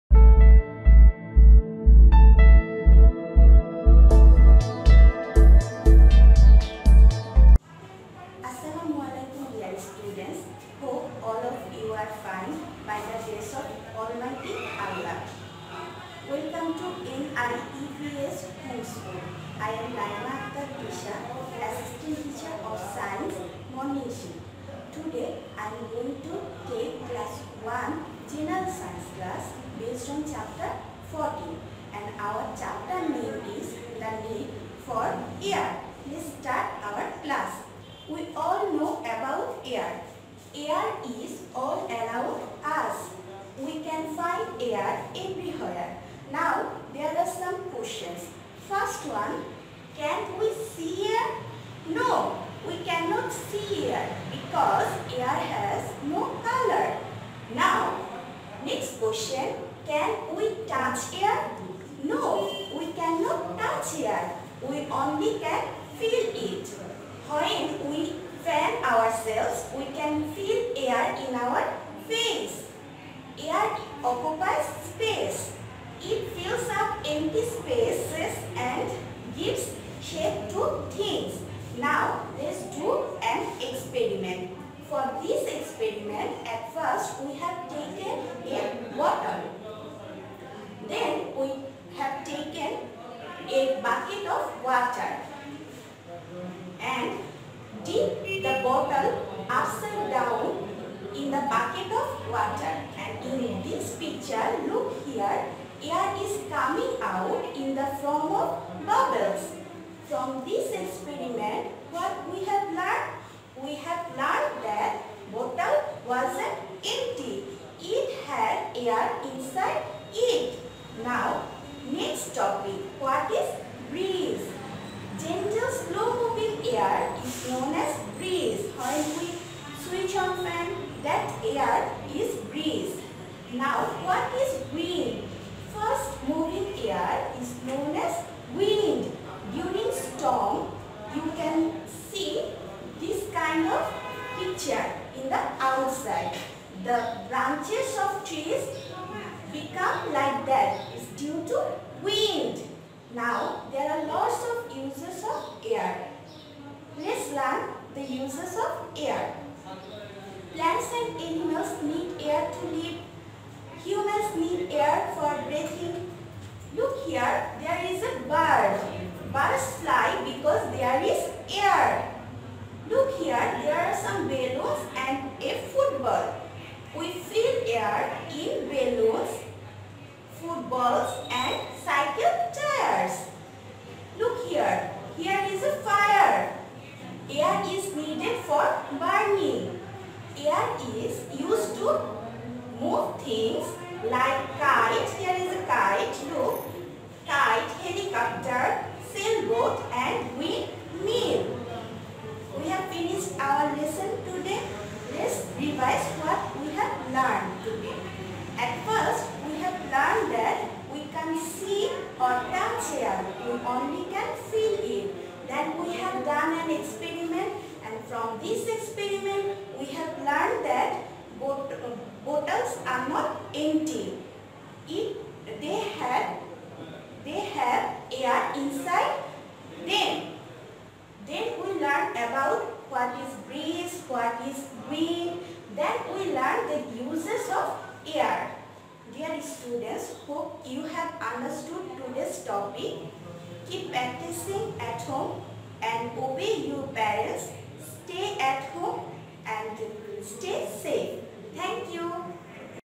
One#General Science
Class: One
Subject: General Science
Topic: The need for air
Teacher: Naima Akter Tesha